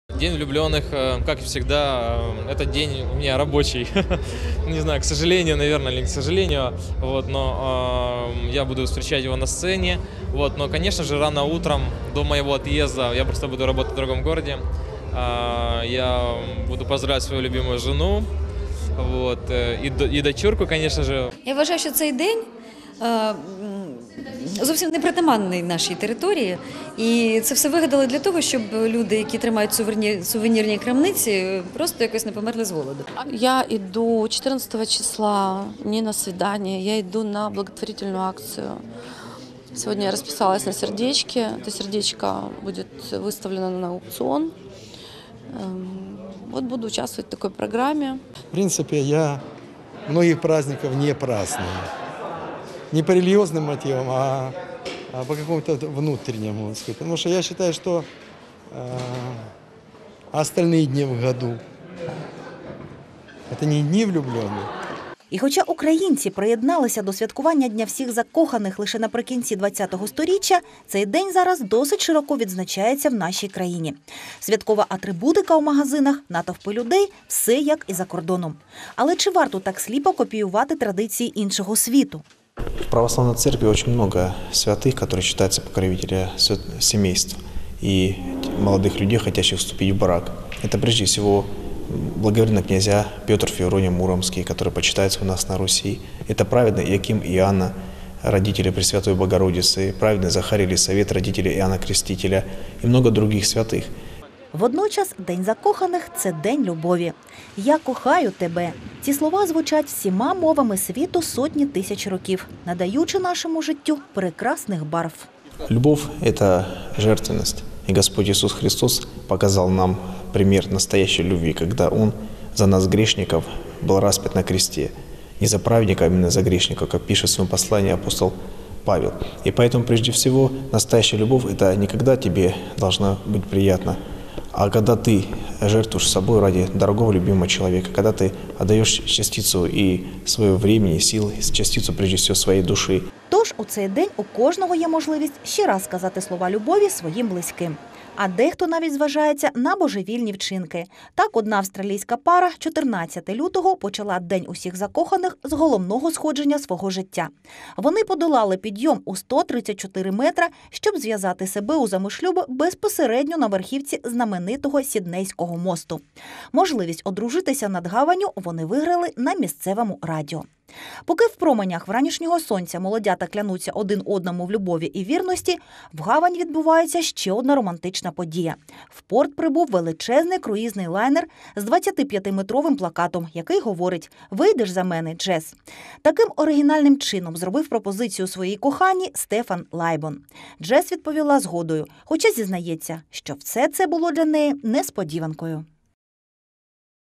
Календарь, любовь и шоу-бизнес
Для России и других стран, входивших ранее в состав бывшей Российской империи, сегодня выдающийся день. Ведь именно 14 февраля 1918 года был осуществлён переход на новый стиль летоисчисления. Тогда сразу после 31-го января наступило - 14-е февраля. Сейчас же, почти век спустя, в этот дату мы отмечаем не переход на новый календарь, а день всех влюбленных. Но стал ли праздник, заимствованный  из Европы и Америки, действительно украинским?  На этот вопрос отвечают  звезды отечественного шоу-бизнеса.